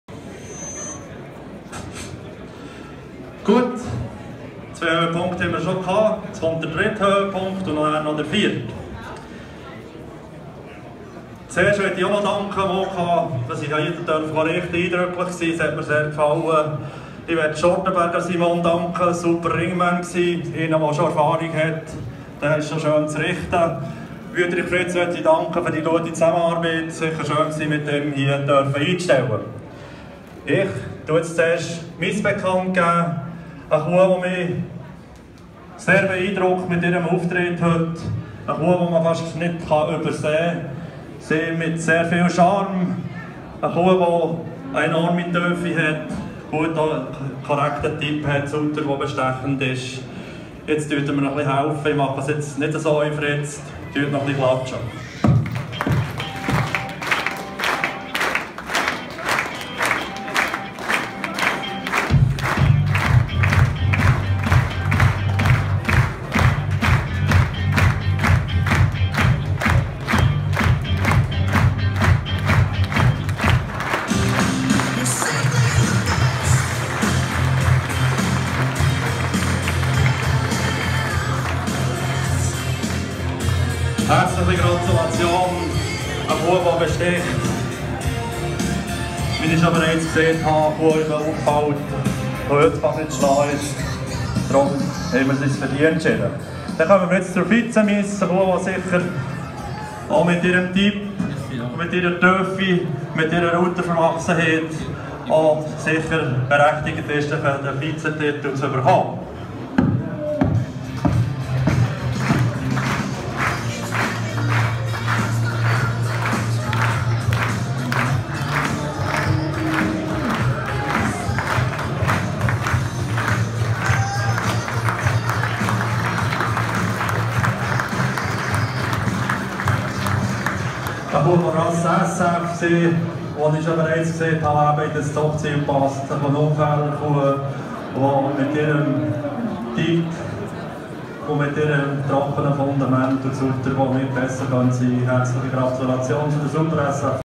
Züchtertreff Riggisberg 2017: Championwahl
Züchtertreff Riggisberg 2017: Miss: Quality Atwood Beauty, Christian Aegerter, Gurzelen. Vizemiss: Wydehof Incas Valencia, Heinz Gilgen, Mamishaus.